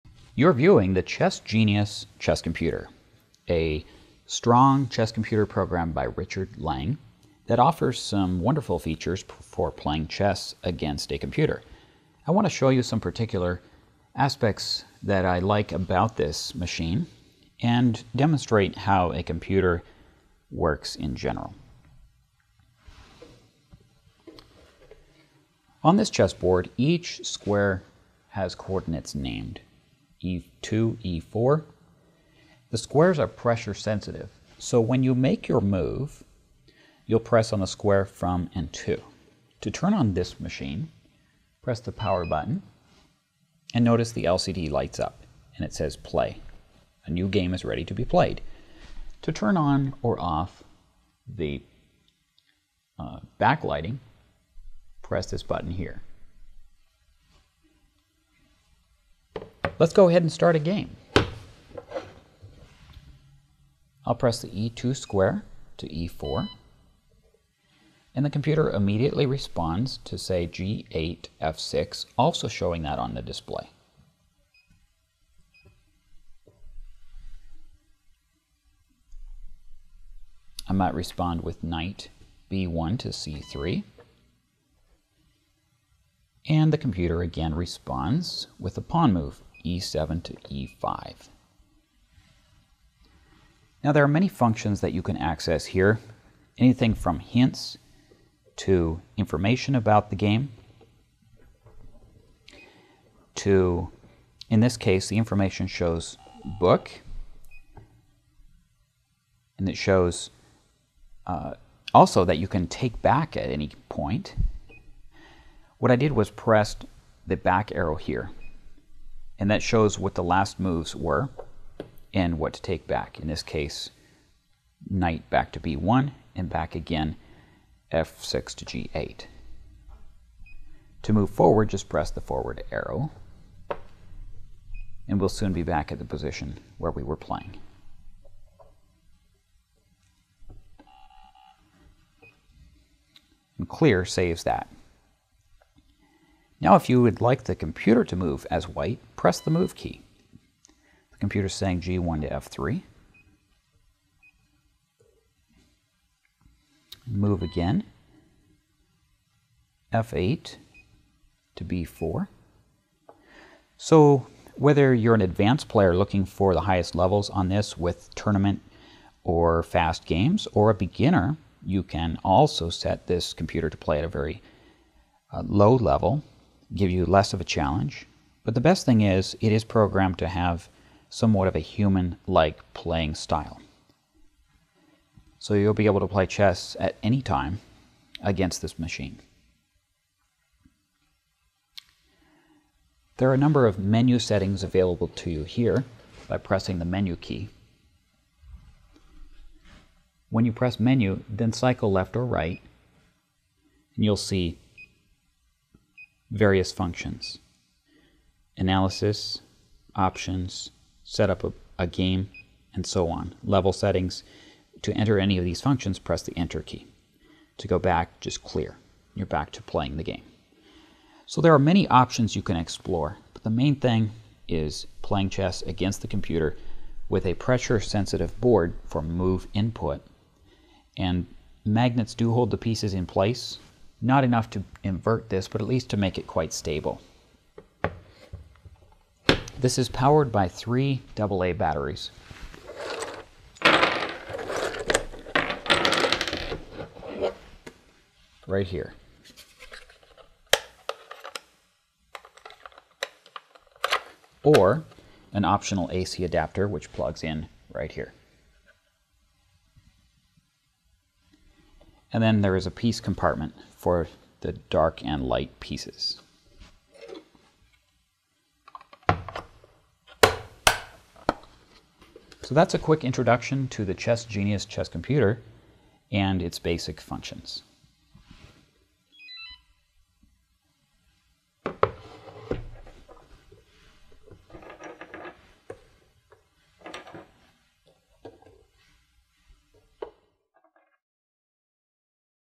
How to Play the Chess Genius Electronic Chess Computer
This chess computer released near the end of 2015 brings current chess programming to the electronic chess scene. And that's what we needed. Now an advanced program can be enjoyed by anyone under Master level play. What this video for an intro to this fun game.